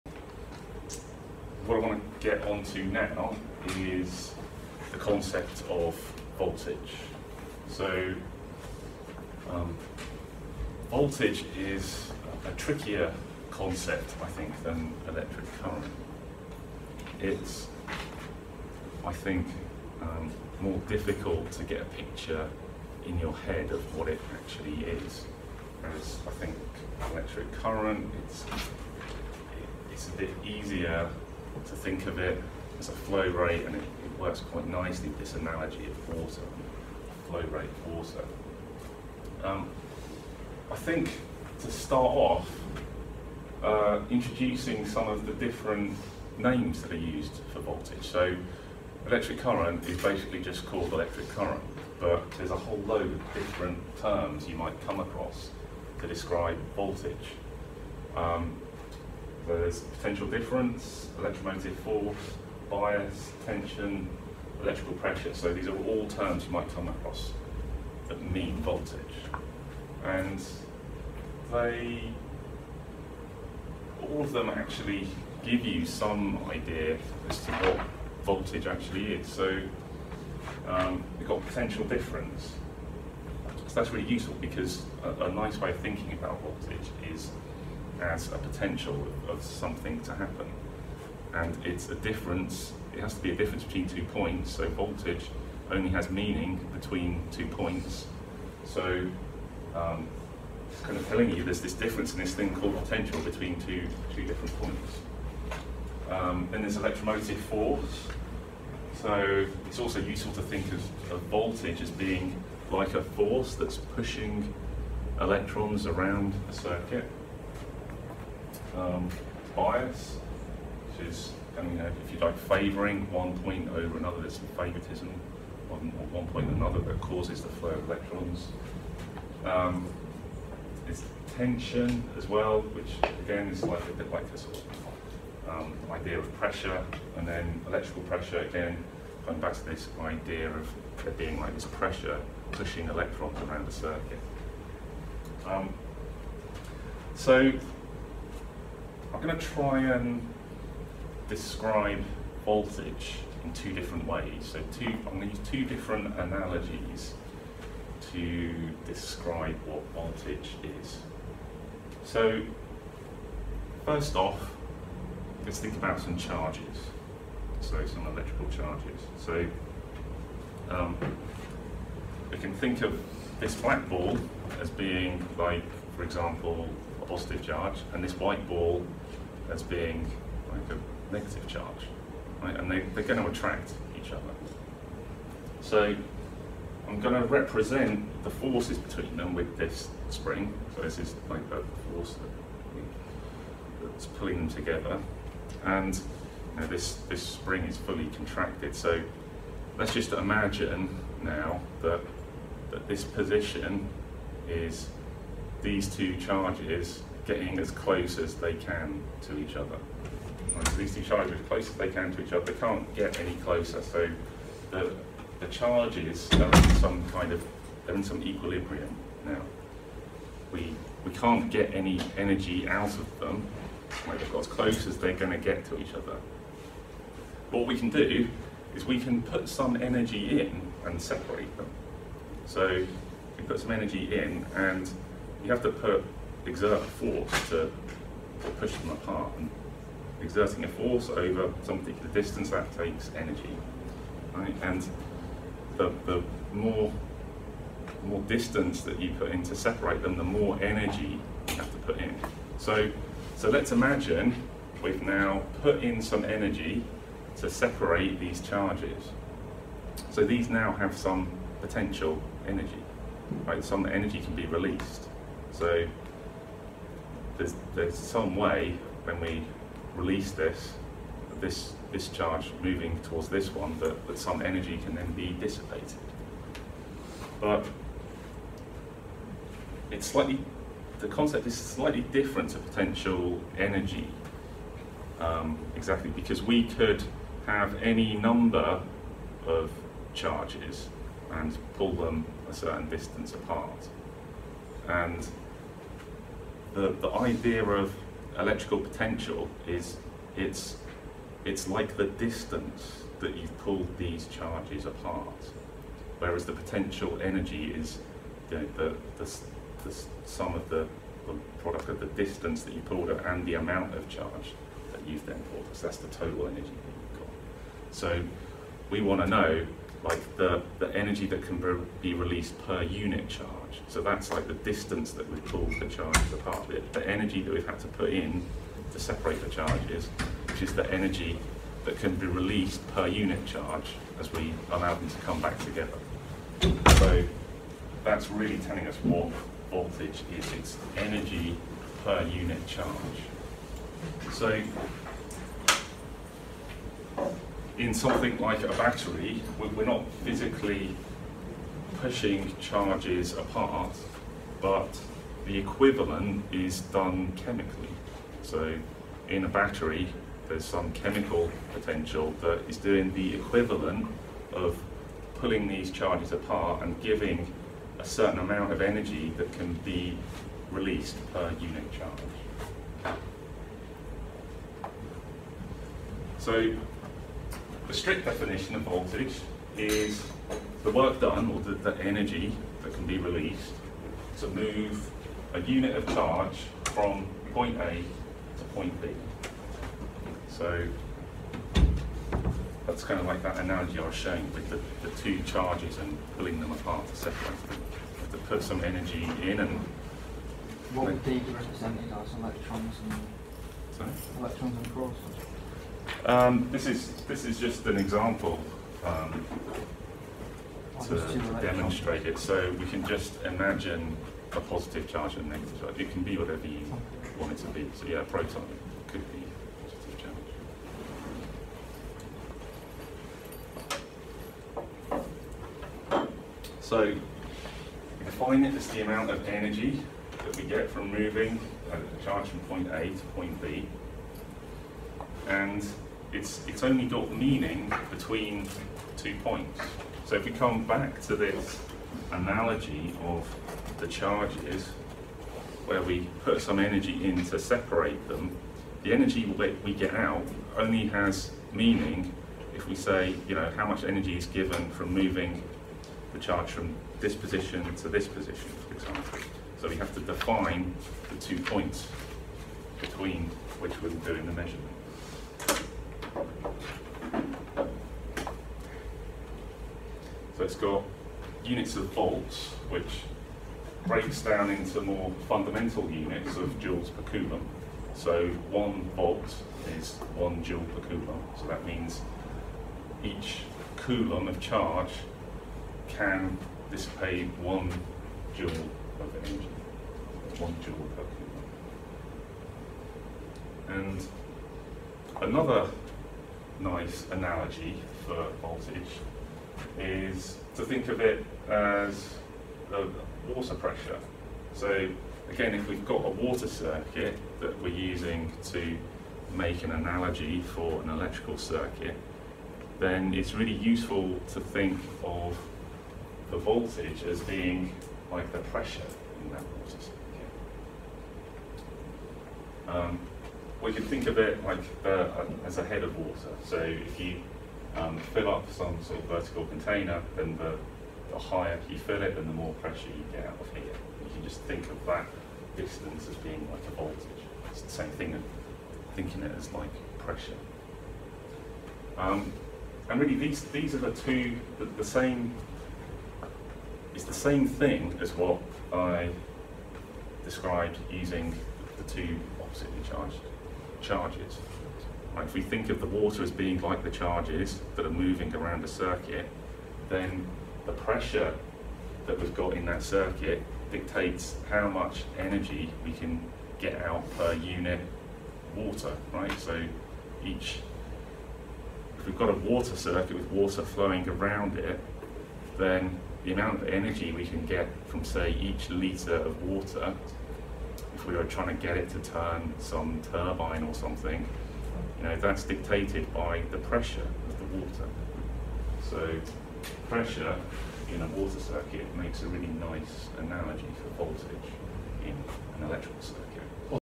What is voltage?
A description of voltage using analogies to water circuits and gravitation